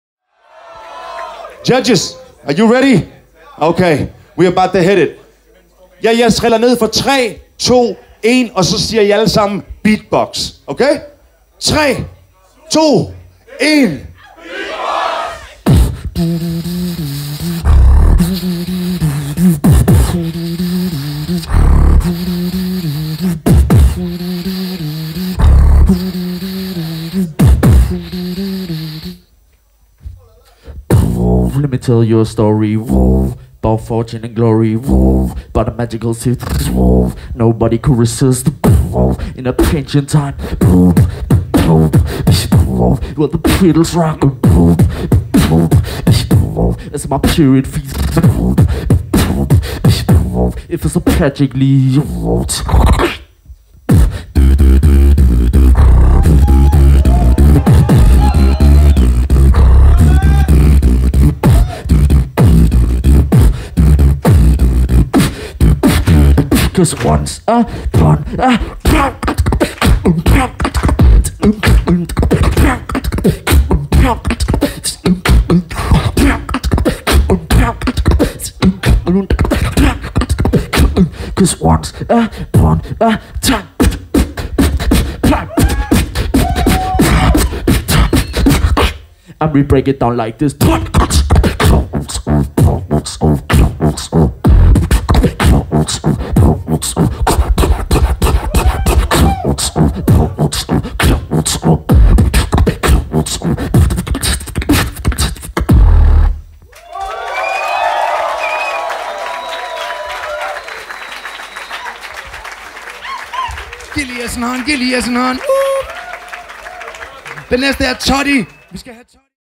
S - Eliminerings runde - DM i Beatbox 2019
DM i Beatbox 2019                     [English below]
S - Eliminerings runde

Dommere - Alexinho (FR), Alem (FR), K.I.M (FR), Eon (AT), The Pounder (DK)
Vært - Simon Adel

Tak til - Odense Kommune, Studenterhus Odense, Roland Danmark, 4Sound og BeadLightStudio

Mange tak til alle deltagere, dommere, publikum, vært, frivillige, kamera hold og fotograf!

Danish Beatbox Championship 2019
S - Elimination

Judges - Alexinho (FR), Alem (FR), K.I.M (FR), Eon (AT), The Pounder (DK)
Host -  Simon Adel

Thanks to - Odense Kommune, Studenterhus Odense, Roland Denmark. 4Sound and BeadLightStudio

Thanks a lot to the participants, judges, audience, host, volunteers, camera team and photographer!

Video editing by Oberes
Audio mastering by Rawclaw

The Danish Championship was organised by Dansk Beatbox Forening aka DBF